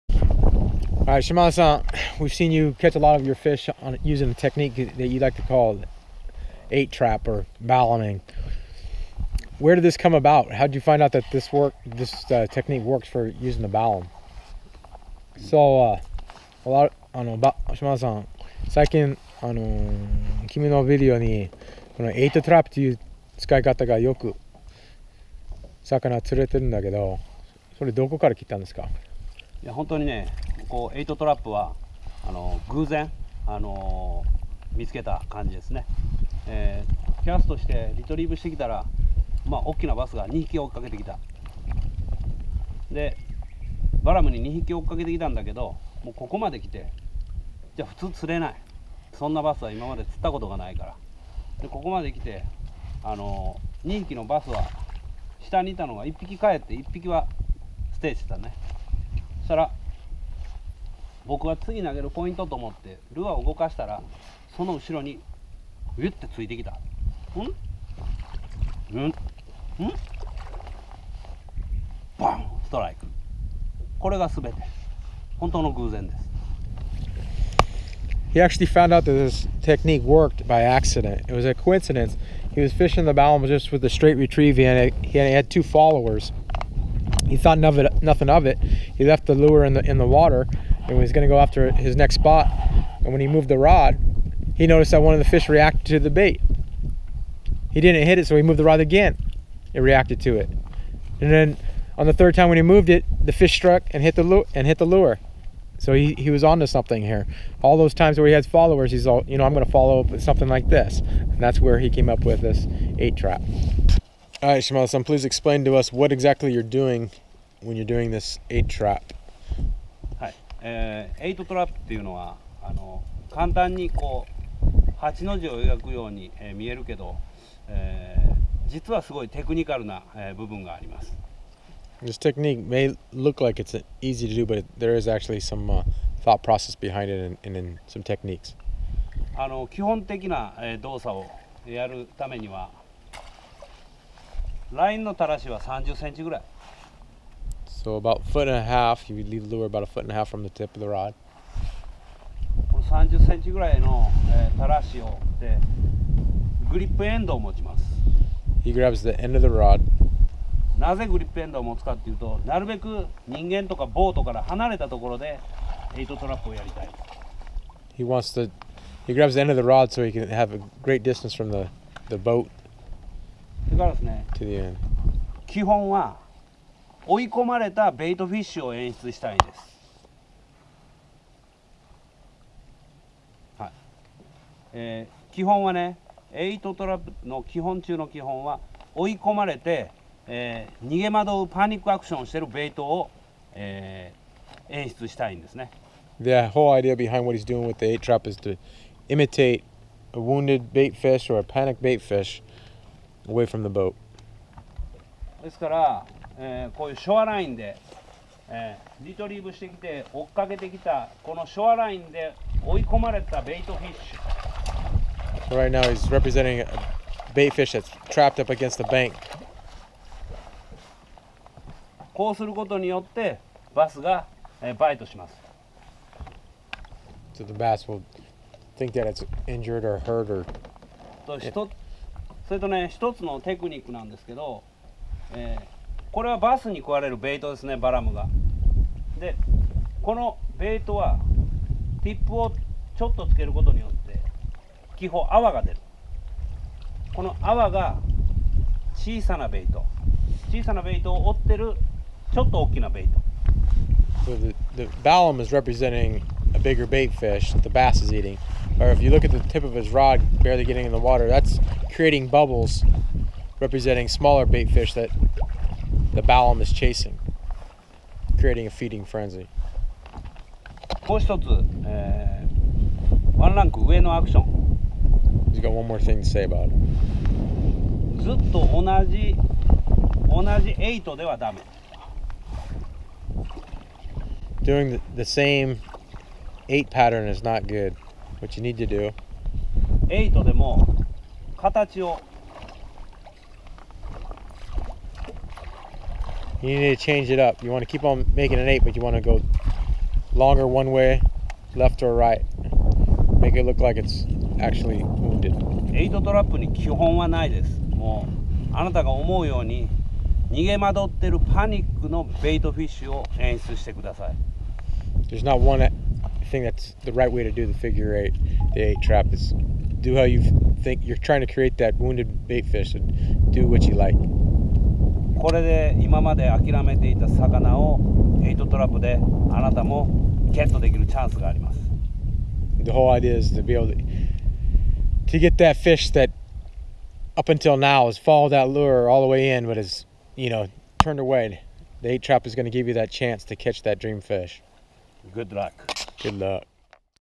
Fishing the 8 Trap Method aka "Balaming" with the Madness Balam 300 Swimbait
Satan Shimada, Lure Designer at Madness Japan, explains the Eight Trap Method for the Balam 300 swimbait.